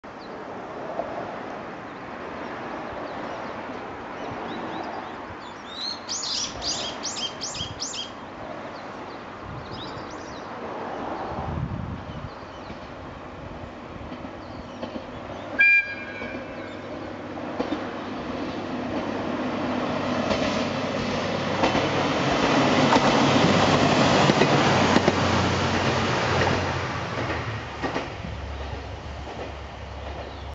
[警笛あり]長良川鉄道 ﾅｶﾞﾗ500形(ﾁｬｷﾞﾝﾄﾝ列車) 前平公園駅付近通過[汽笛吹鳴標識]
長良川鉄道ﾅｶﾞﾗ500形(ﾁｬｷﾞﾝﾄﾝﾗｯﾋﾟﾝｸﾞ車)「美濃市行き」の前平公園駅付近通過シーンです。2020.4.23撮影
付近に第4種踏切があり、その手前に警笛吹鳴標識が設置されている為警笛が鳴ります。
1再生リスト200本・33シリーズまで作成済